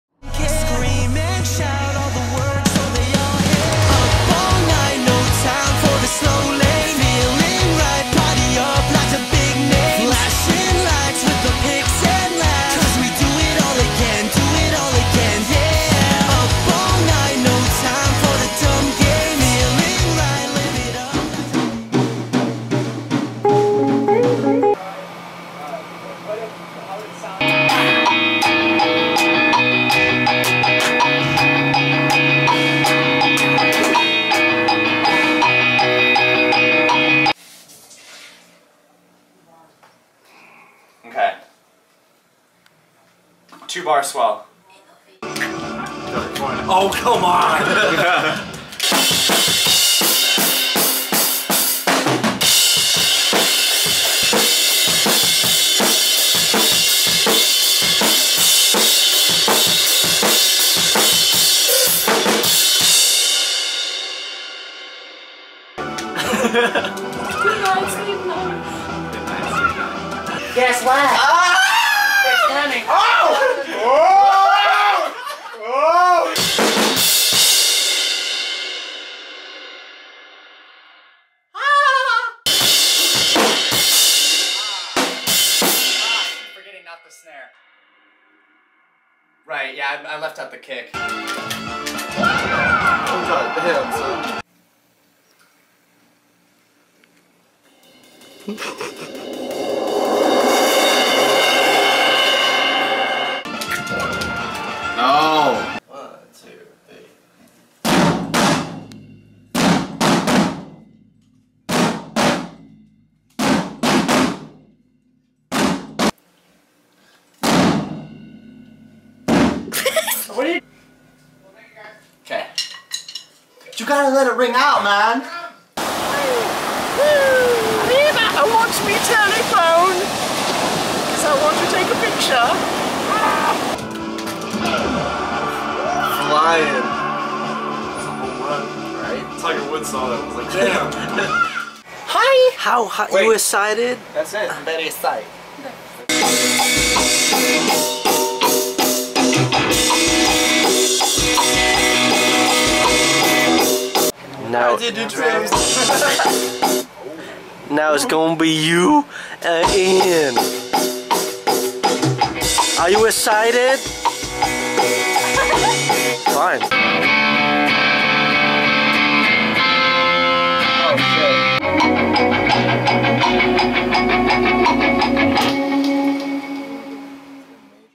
Smile Asterisk Studio Update #1 | Drums-N-Games
Studio Update #1, Drums! Check out Josh laying down some drums on the EP! Also, we have a few behind-the-scenes clips of what we do on our breaks!

(INSTAGRAM) SmileAsterisk